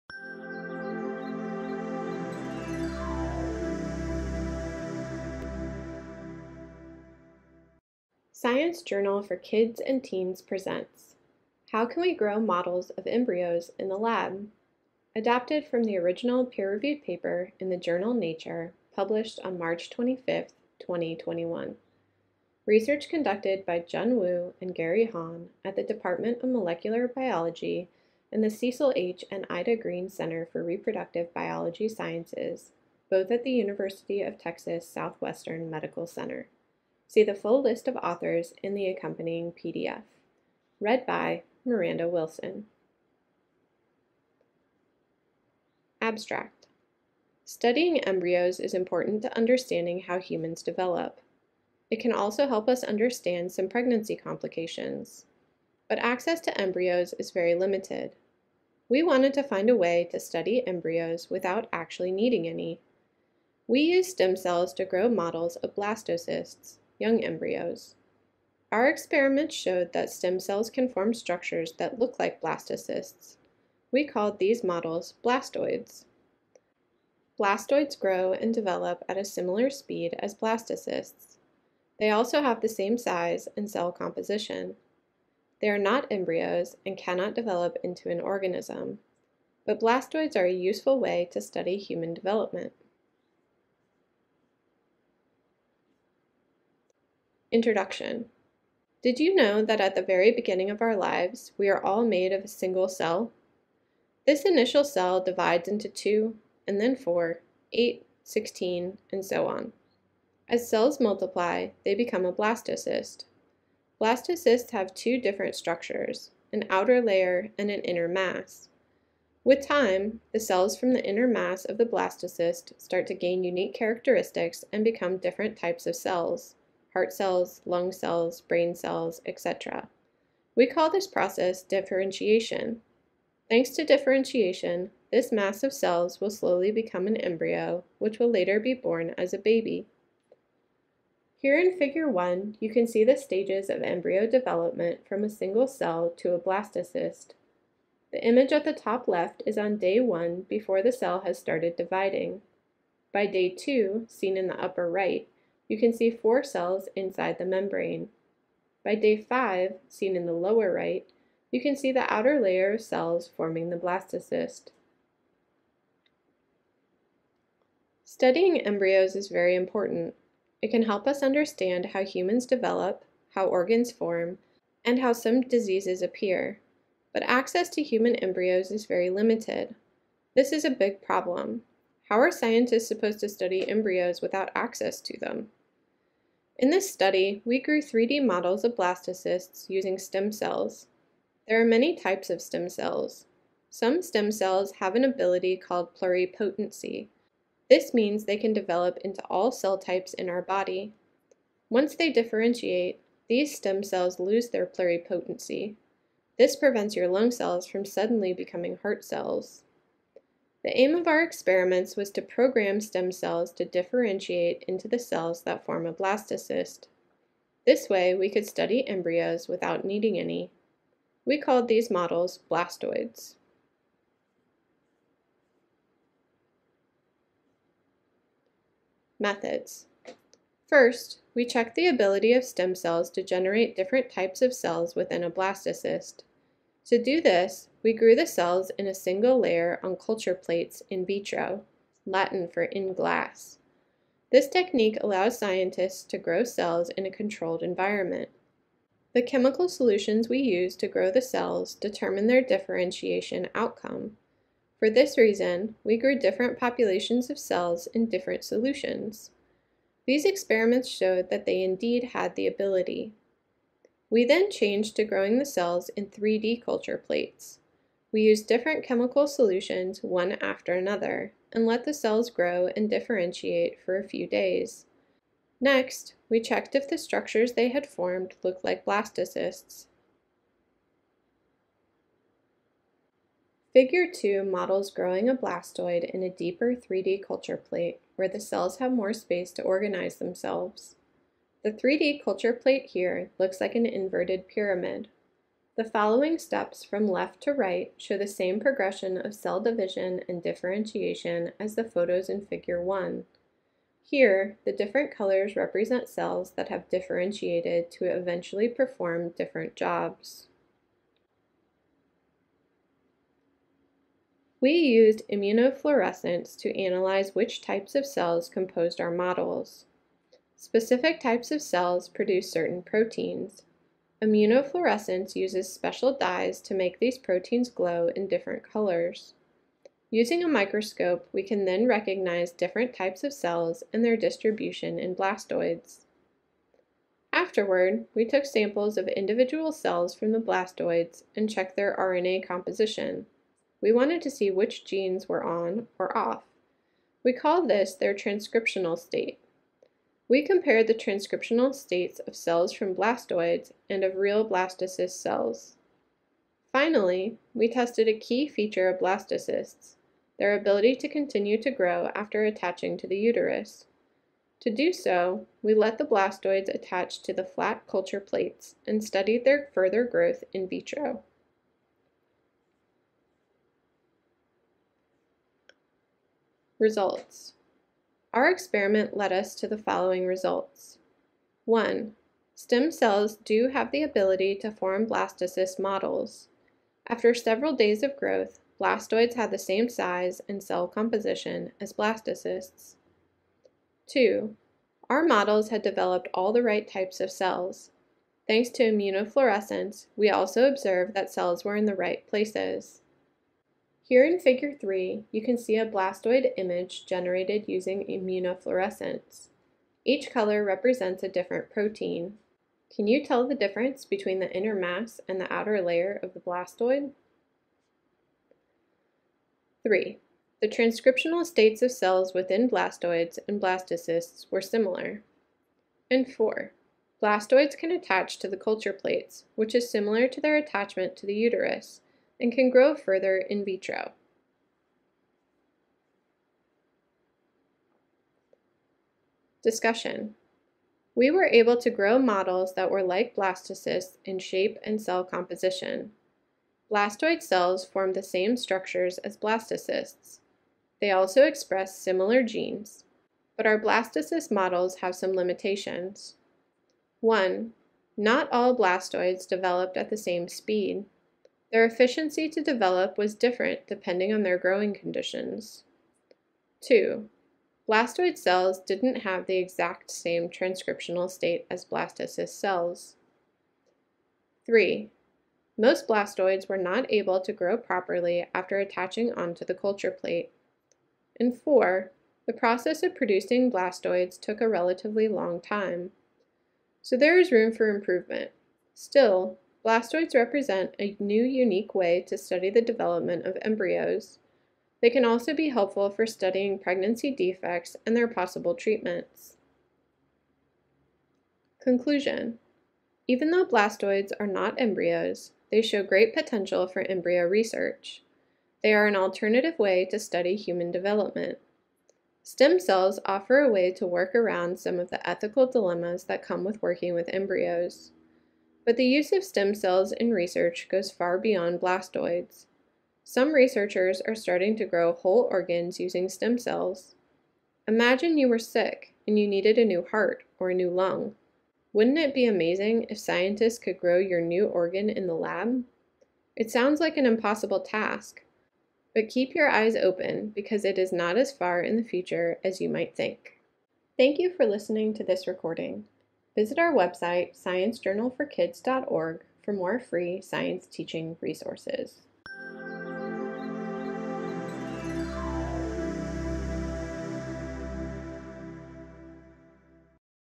E29: How can we grow models of embryos in the lab? [SJK Audio Edition]
Summary:
Scientists used stem cells to grow 3D models of blastocysts to be able to study the development of embryos.

Abstract
Studying embryos is important to understanding how humans develop. It can also help us understand some pregnancy complications. But access to embryos is very limited. We wanted to find a way to study embryos without actually needing any! We used stem cells to grow models of blastocysts – young embryos. Our experiments showed that stem cells can form structures that look like blastocysts. We called these models blastoids.

Blastoids grow and develop at a similar speed as blastocysts. They also have the same size and cell composition. They are not embryos and cannot develop into an organism. But blastoids are a useful way to study human development.